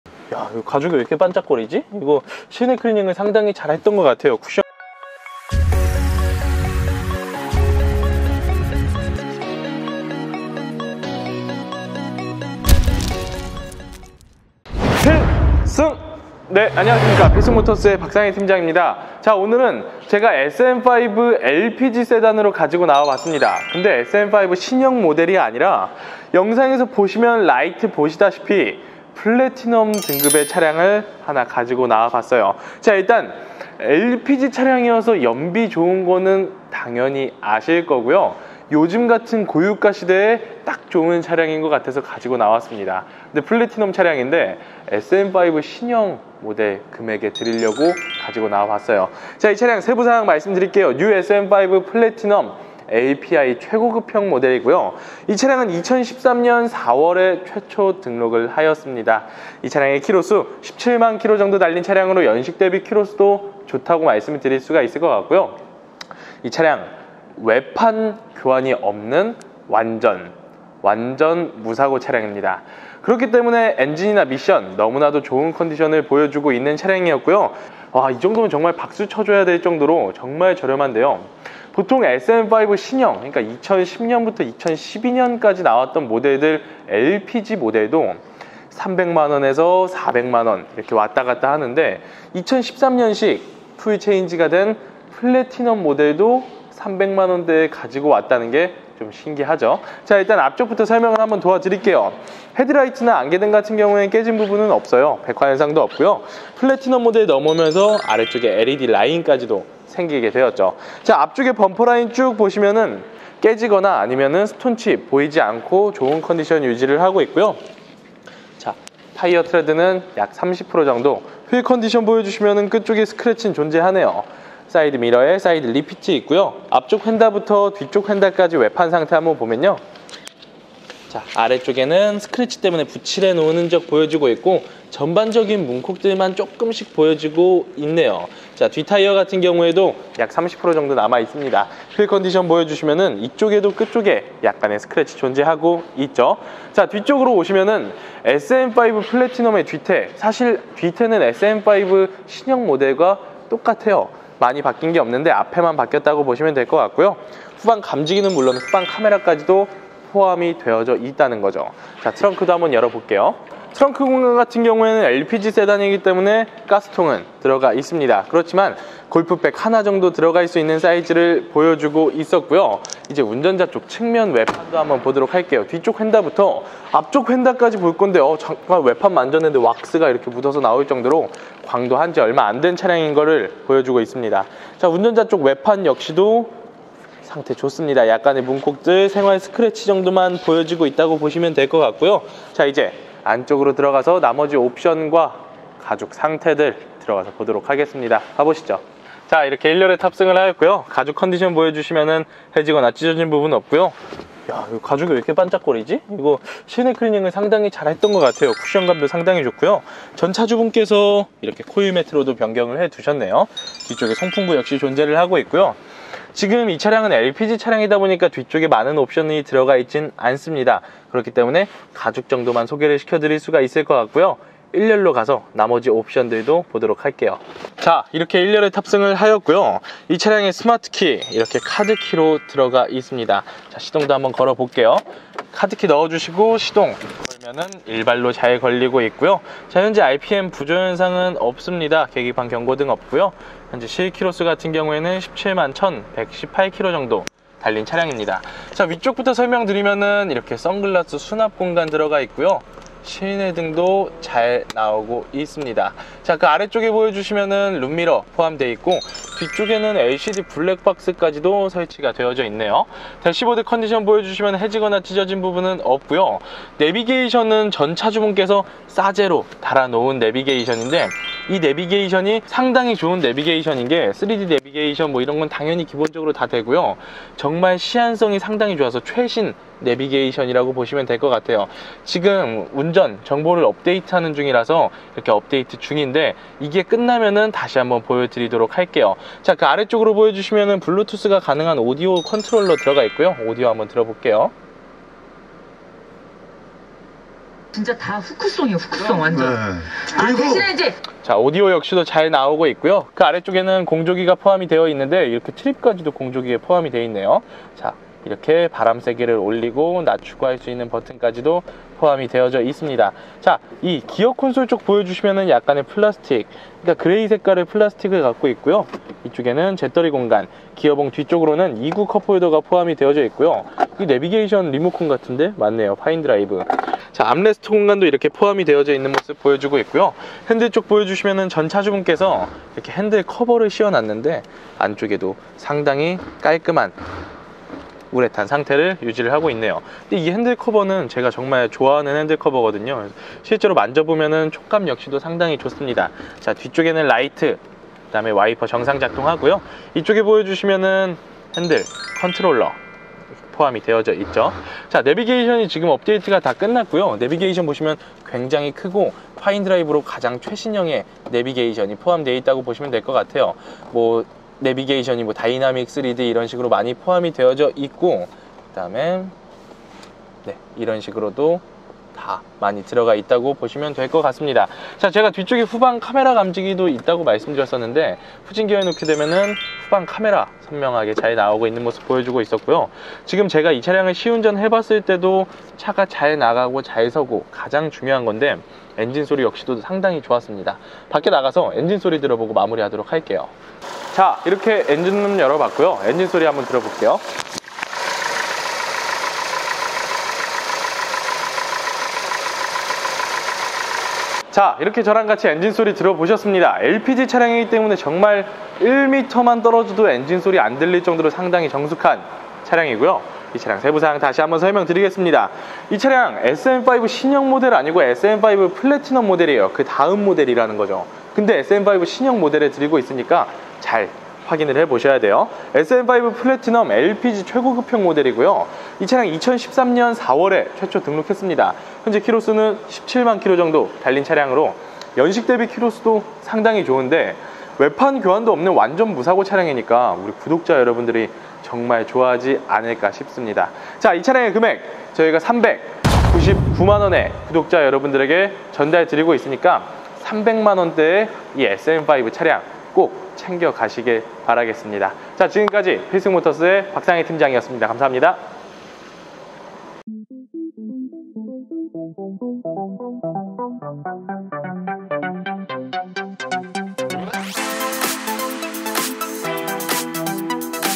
요즘 LPG차 인기가 많네요, 그래서 준비했습니다. 삼성에 중형세단 [SM5 플래티넘]
100% 실매물로 영업하고 있는 필승모터스 입니다.